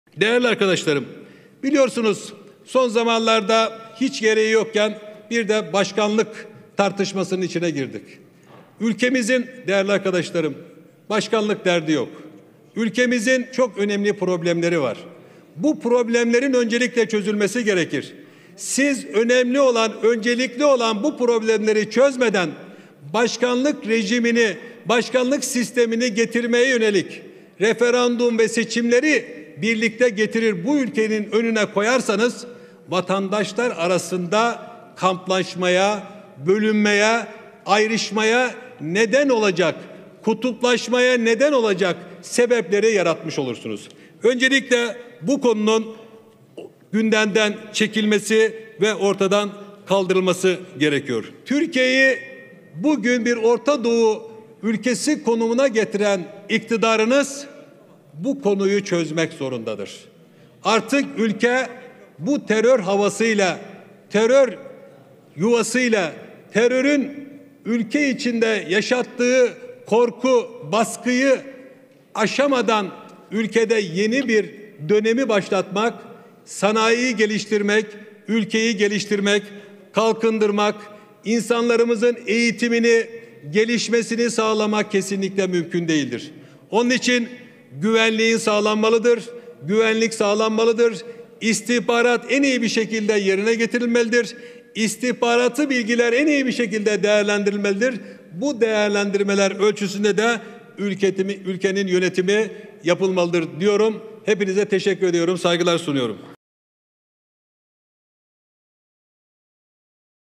Ülkede Başkanlık Değil Terör Sorunu Var!
Denizli Milletvekilimiz Kazım Arslan: Siz öncelikli olan problemleri çözmeden başkanlık rejimini getirmeye yönelik referandum ve seçimleri birlikte bu ülkenin önüne koyarsanız; vatandaş arasında kamplaşmaya, bölünmeye, ayrışmaya, kutuplaşmaya neden olacak sebepleri yaratmış olursunuz.